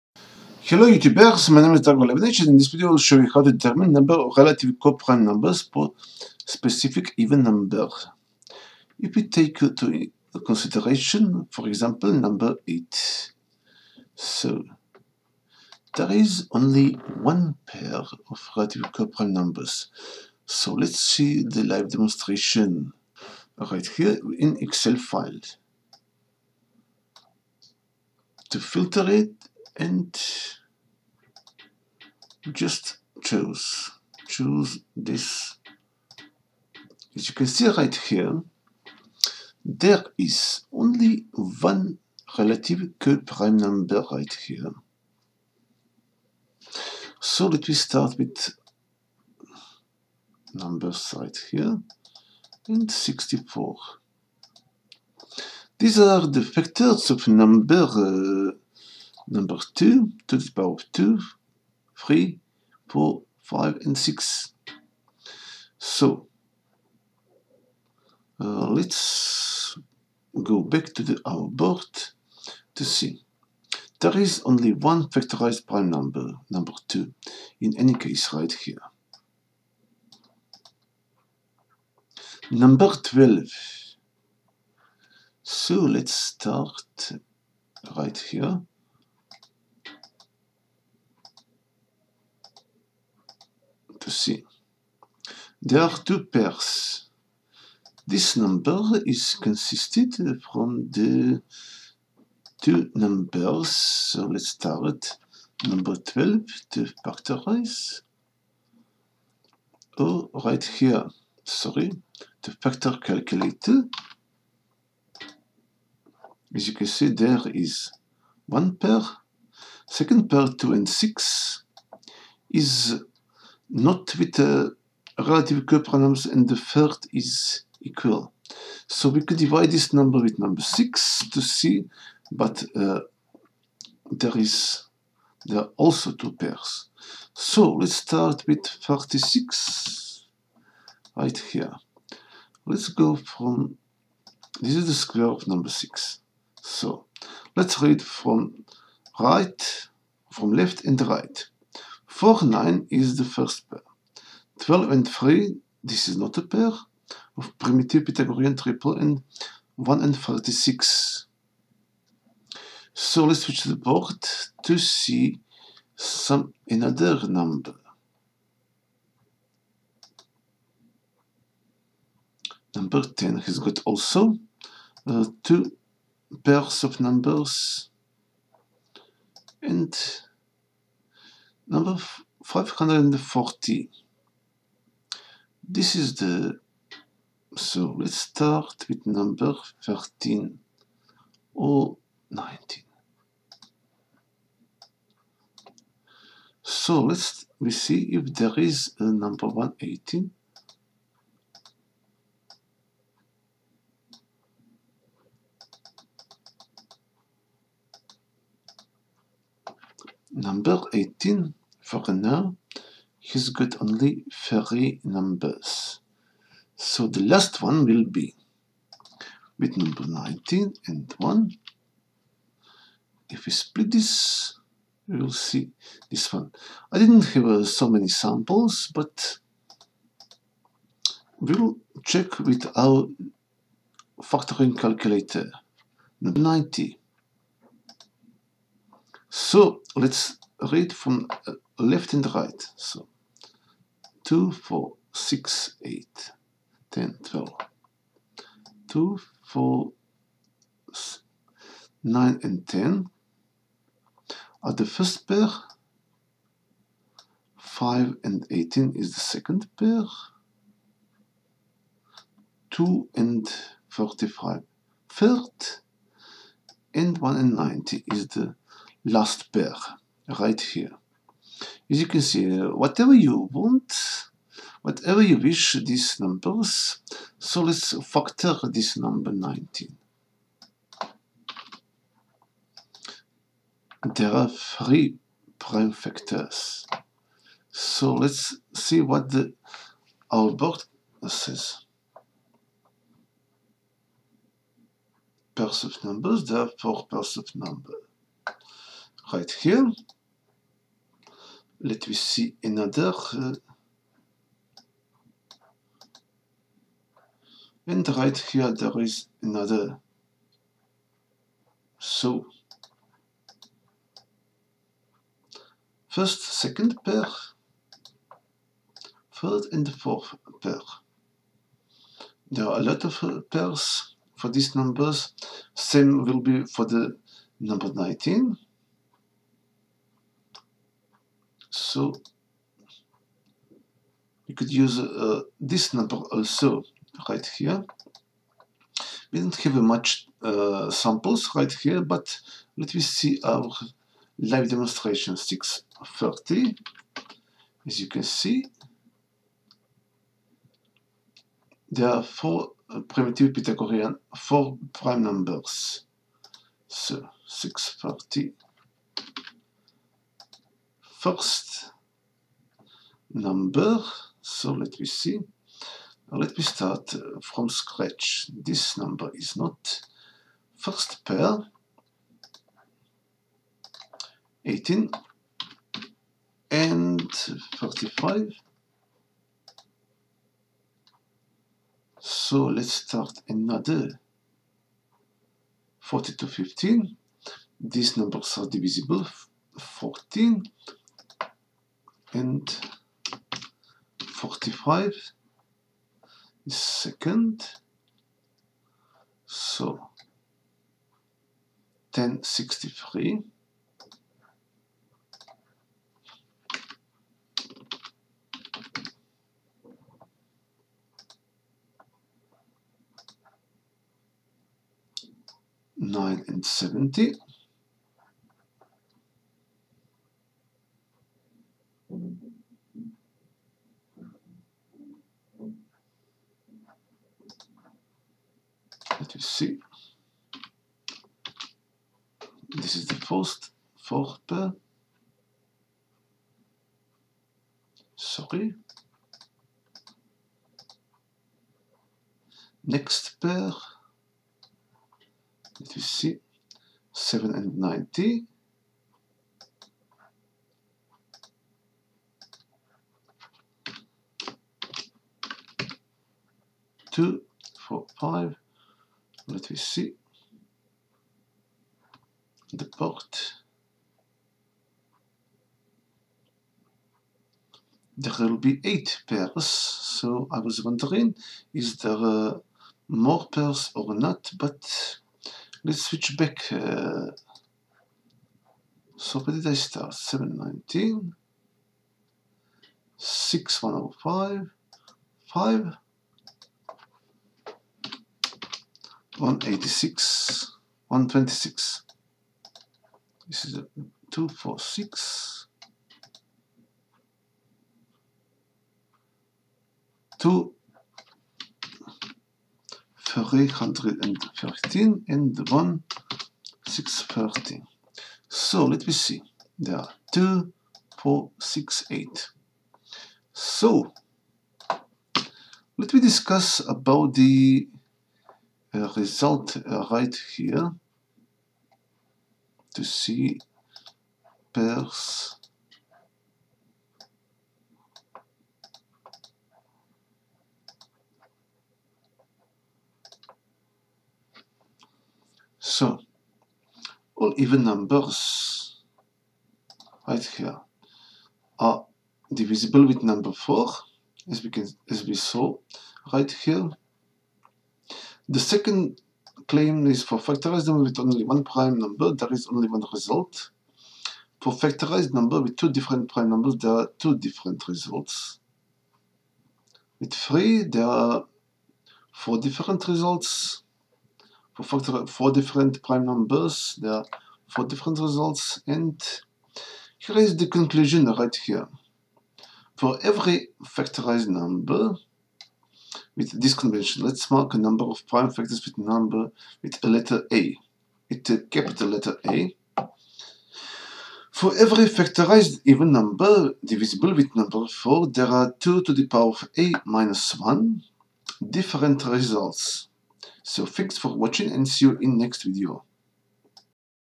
Primitive pythagorean triples even number
In this video it is described how to find a smaller odd number and how many times will occur any odd number depending on the number of factors. This is extended version of following videos Tools I have been used are: microsoft excel, number factorizer and factorization calculator